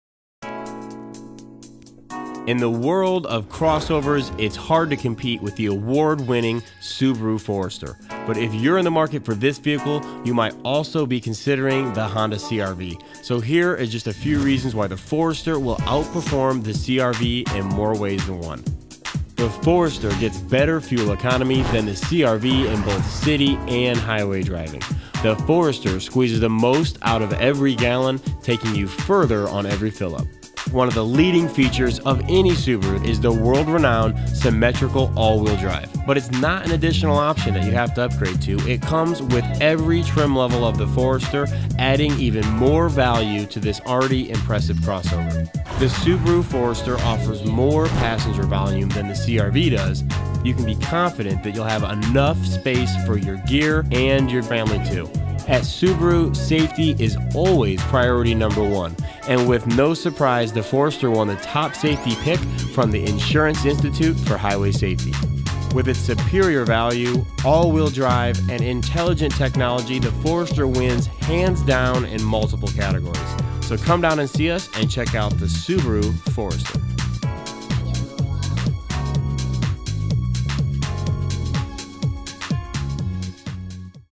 2014 Subaru Forester vs Honda CRV - Stohlman Subaru Tysons Corner
Why buy a 2014 Subaru Forester? Better fuel economy, standard AWD, more passenger and cargo space, and a top safety pick! Stop by Stohlman Auto in Tysons Corner for more details and to see the difference for yourself!